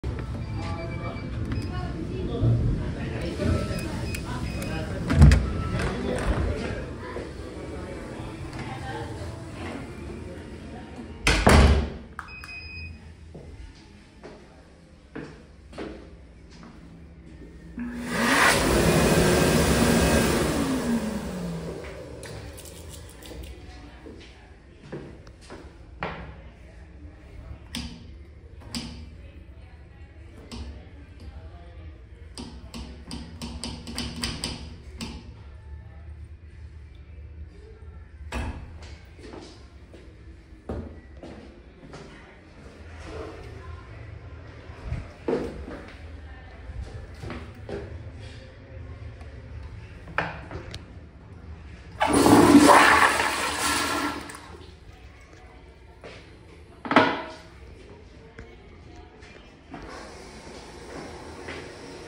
Panda Express Unisex Restroom Full Shoot!
This was filmed off Elverta Road in Sacramento, CA yesterday.

This place was renovated between 2022 and 2024 because of how new it looked, and it once had evil restrooms. Now it’s two unisex restrooms with passcodes.

The other one I tried to film, but it was coded too with a different combination, but after what I saw in the one I filmed….